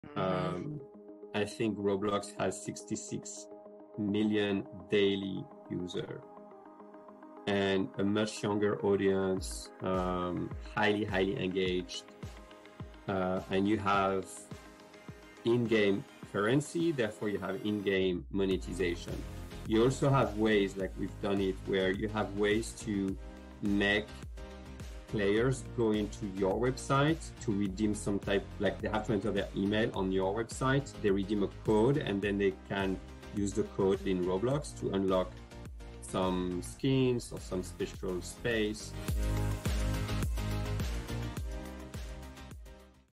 XR Pub Crawl June Highlight: In-game Brand Experiences
Roblox, Fortnite, Minecraft and others are popular virtual gaming platforms with MILLIONS of daily users, especially among the younger generation. They feature in-game currency, monetization and ways for players to redeem codes on websites to unlock exclusive skins and special spaces.